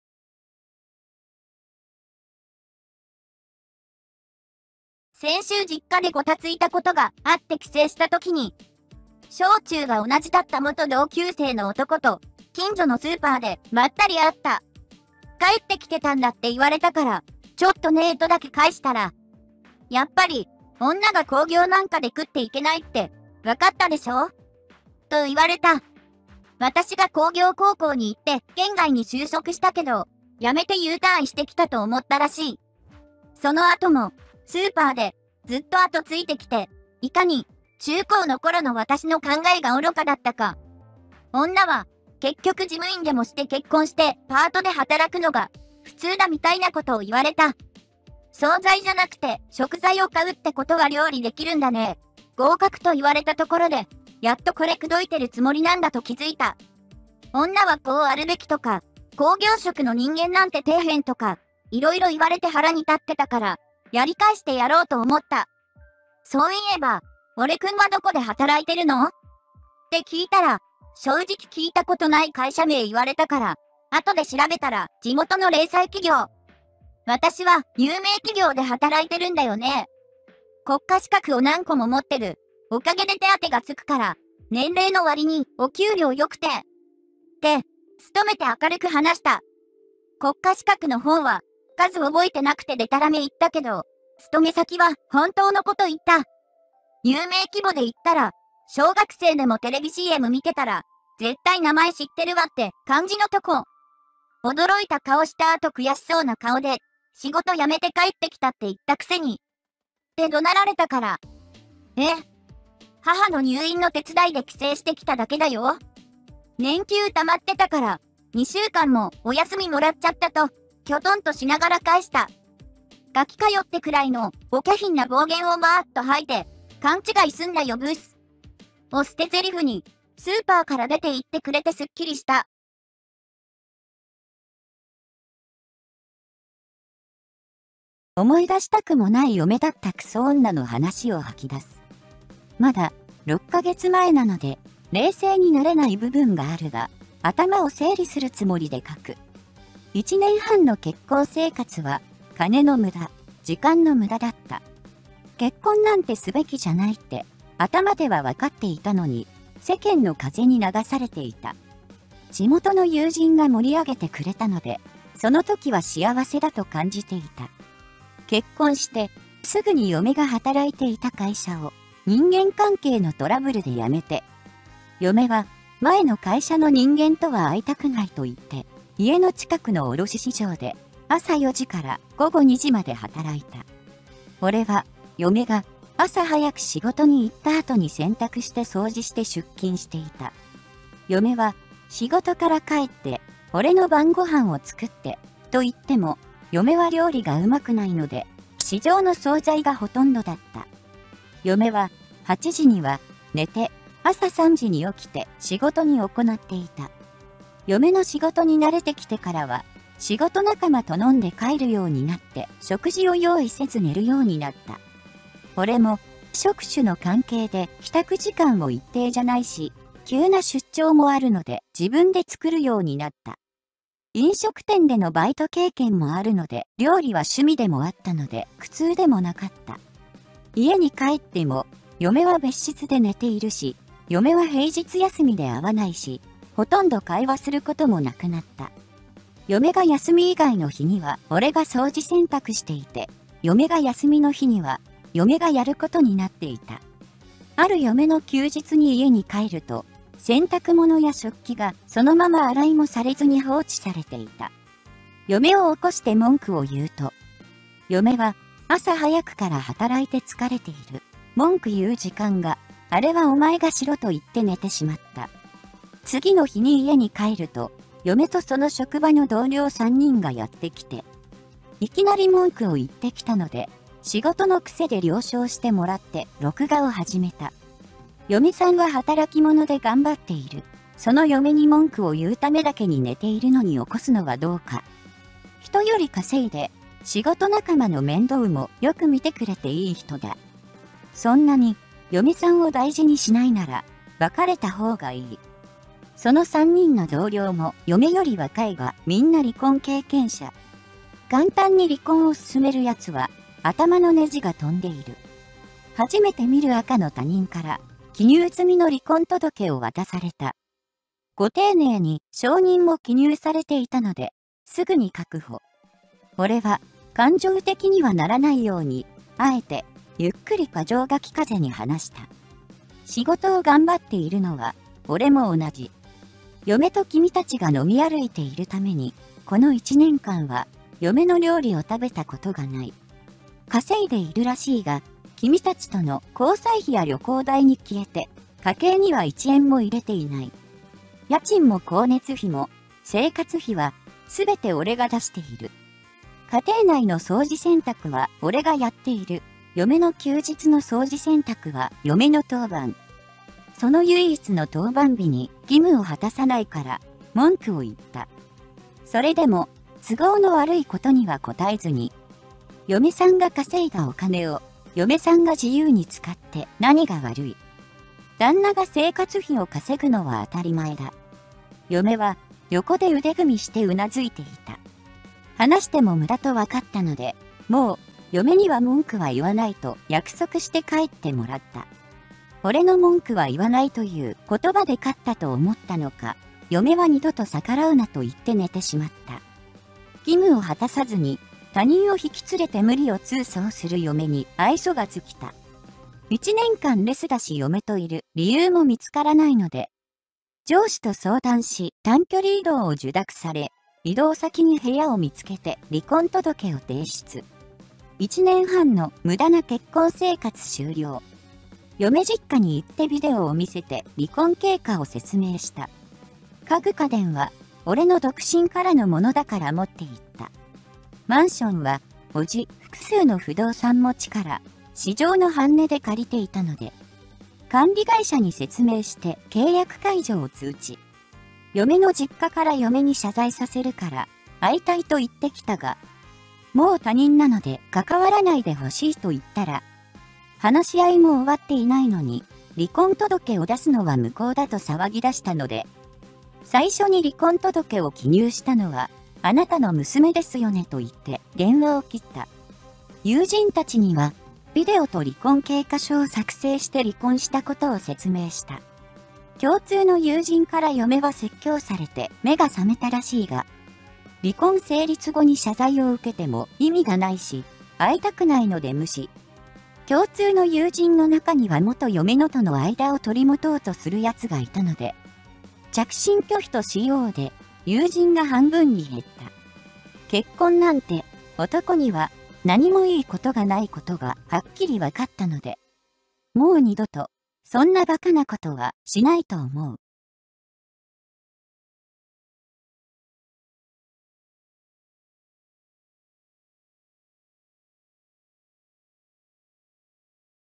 帰省中、スーパーで元同級生の男と遭遇。男「女が工業で食っていけないって分かったでしょ？」私「は？有名企業で働いてるよｗ」…【痛快・スカッとジャパン】
※動画音楽
　LAMP BGM　様

0:00　帰省中、スーパーで元同級生の男と遭遇。男「女が工業で食っていけないって分かったでしょ？」私「は？有名企業で働いてるよｗ」…

2:13　【修羅場】１年半の結婚生活は金の無駄、時間の無駄だった。嫁に文句を言ったら、職場の同僚を連れてきた。同僚「嫁さんは働き者だ！大事にしろ！」　#